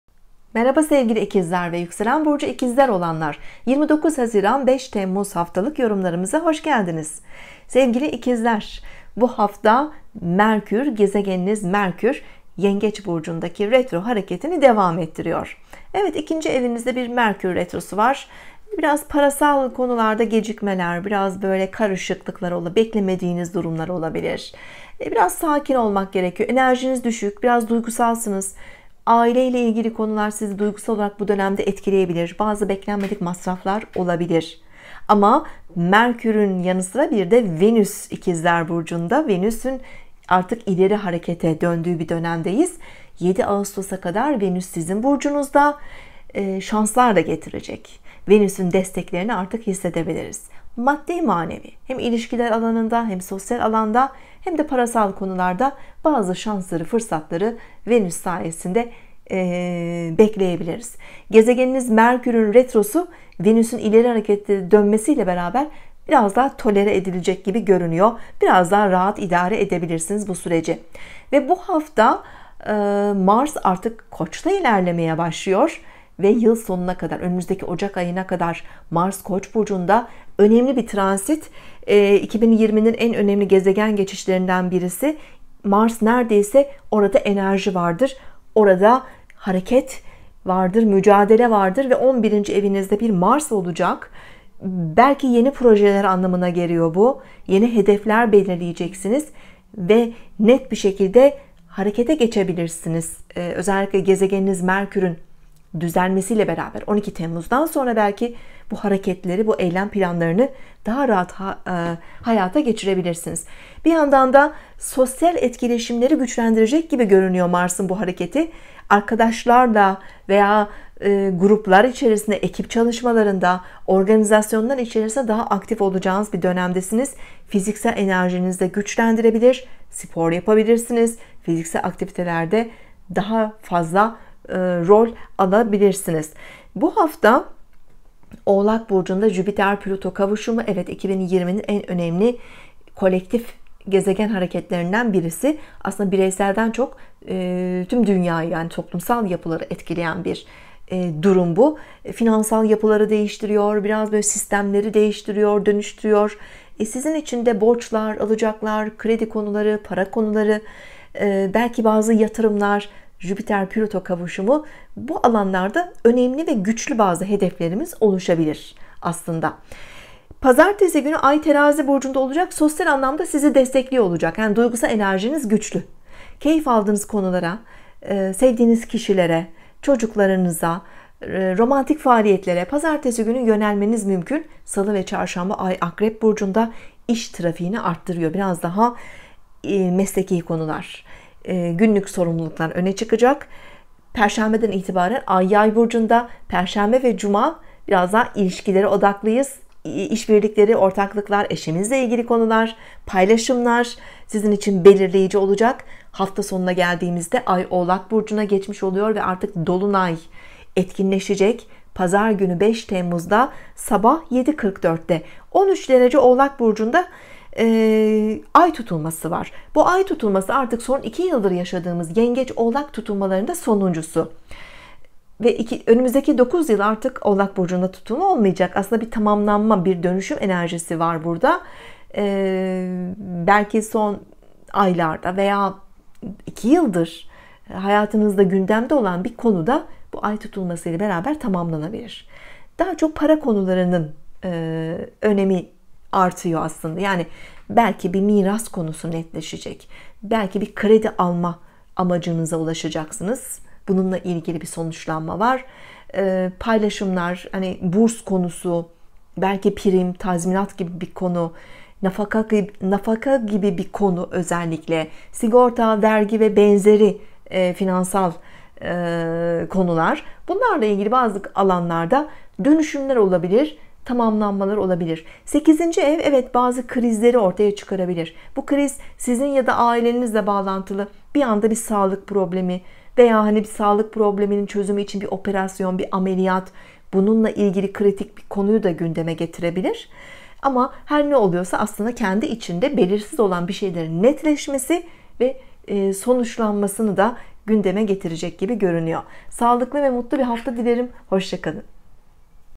Haftalık Burç Yorumları, ikizler Burcu, 29 Haziran 2020 (Astroloji)
29 haziran - 5 Temmuz 2020 Haftalık Burç Yorumları (ikizler Burcu)
ikizler Burcu 22-28 Haziran 2020 haftası sağlık, iş, kariyer ve genel  haftalık yorumları (Astroloji)